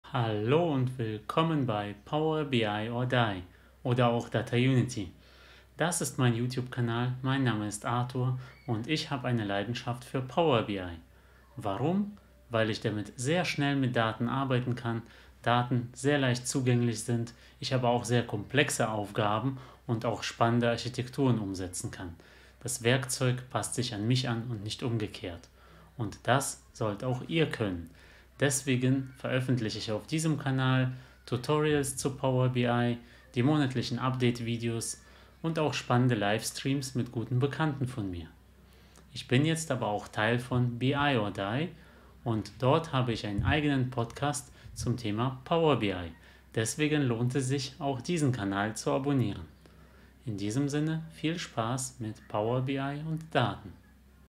Was erwartet euch auf diesem Kanal?
Auf diesem Kanal zeige ich, wie Daten mit Power BI mehrwertbringend genutzt werden können.
Dabei kann der Start sehr einfach sein, um z.B. bestehende Excel-Routinen abzulösen. Aber auch komplexe Datenmodelle sind möglich - Power BI bedeutet BI und Datennutzung für alle...!

Da Power BI fast jeden Monat neue Funktionen und Updates erhält, ist es sinnvoll, sich regelmäßig damit zu beschäftigen und permanent dazuzulernen - was ich hier in meinen Kommentaren machen.

Auch Live-Streams und der Austausch mit der Community sind mir wichtig, so dass auch solche Formate hier Platz finden.

Viel Spaß mit Power BI und mit Daten..!